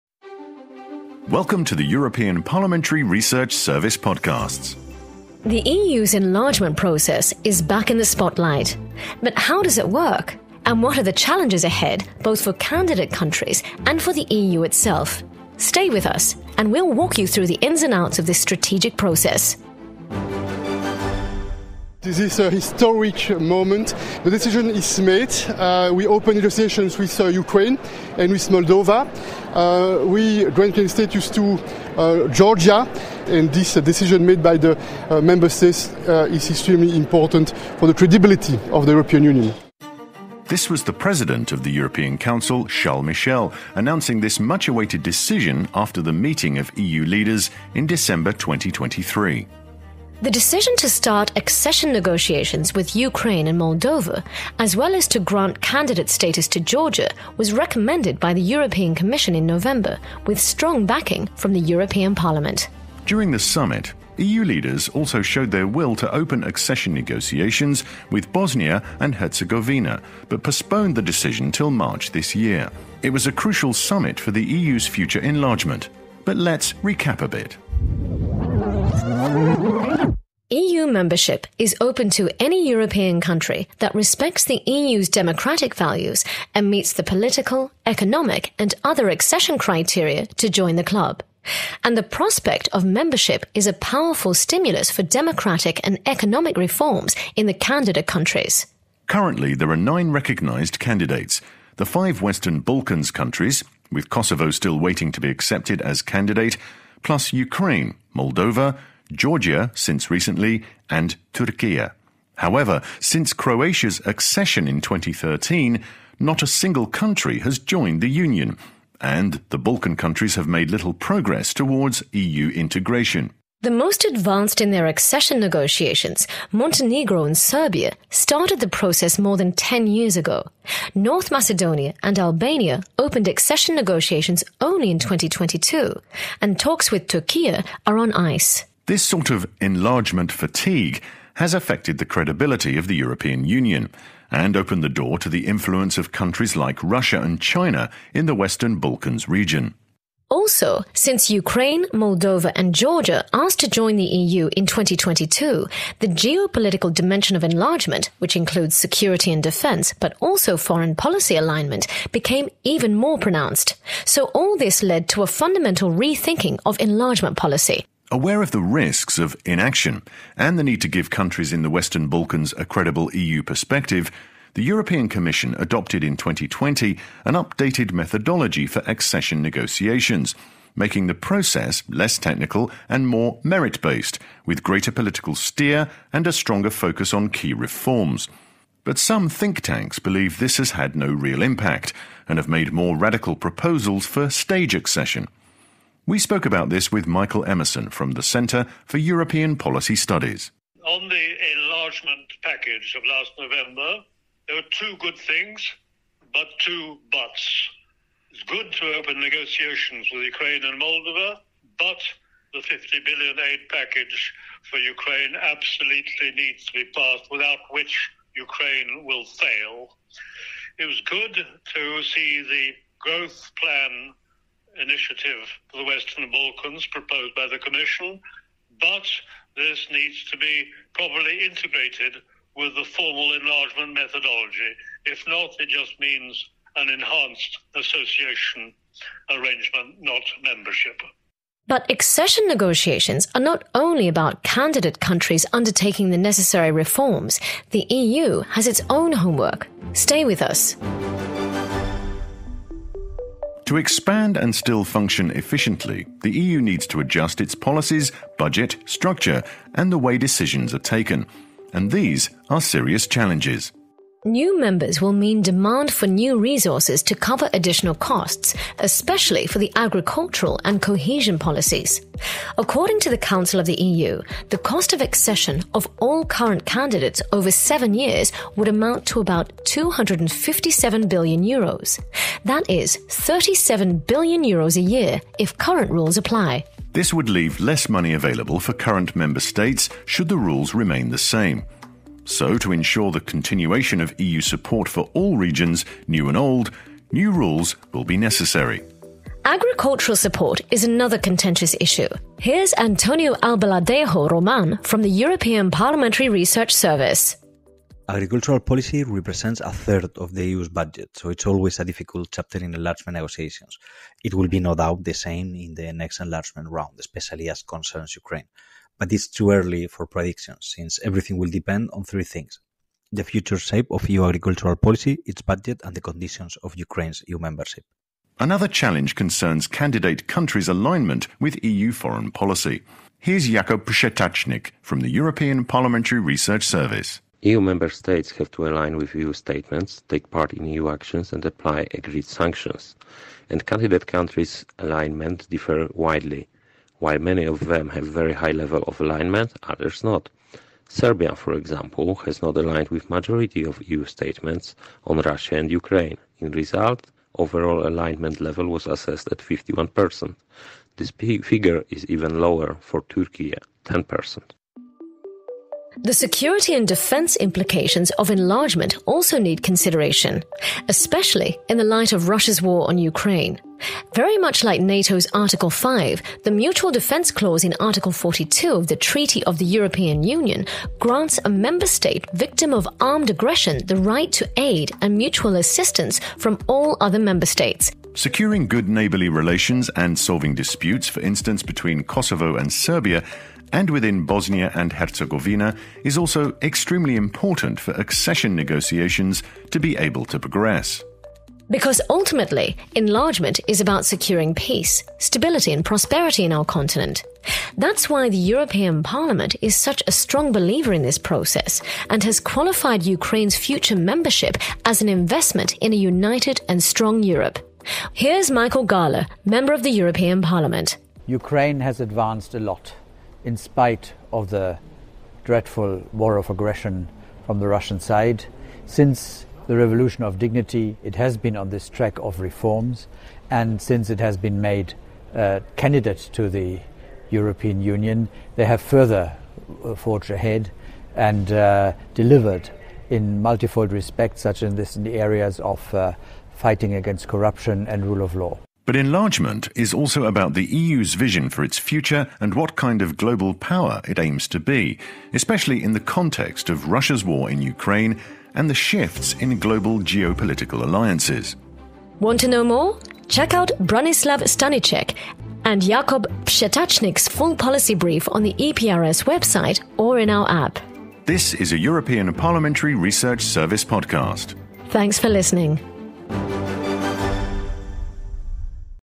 Enlargement policy: Reforms and challenges ahead [Policy Podcast]
In November 2023, the European Commission presented its annual enlargement reports. Its strategic orientations and recommendations include accelerating the accession process with the Western Balkan countries, opening accession negotiations with Ukraine and Moldova, and granting candidate status to Georgia. According to the European Parliament's 2022 recommendation on the new EU strategy, enlargement policy is 'the most effective EU foreign policy instrument and one of the Union's most successful policies'.